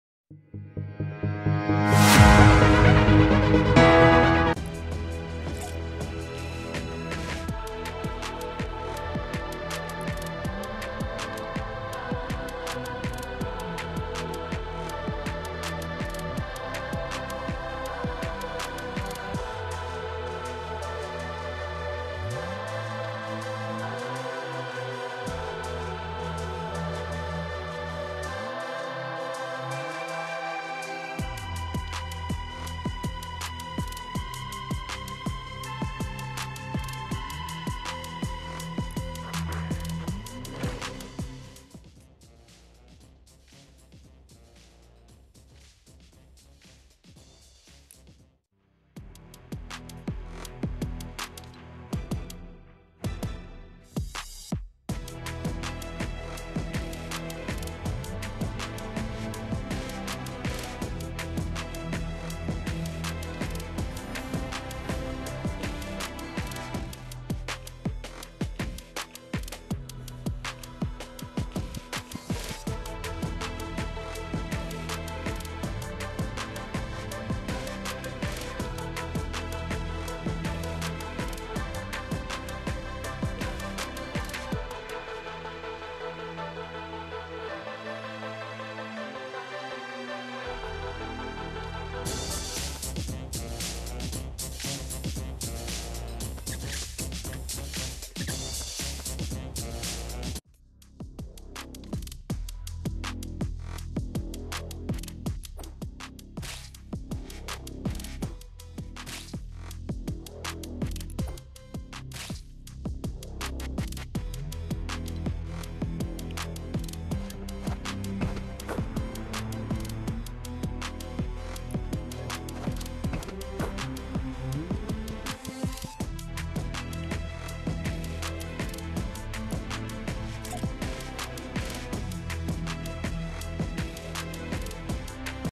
30th May item shop
Here is the item shop for the 30th May containing the hang time set.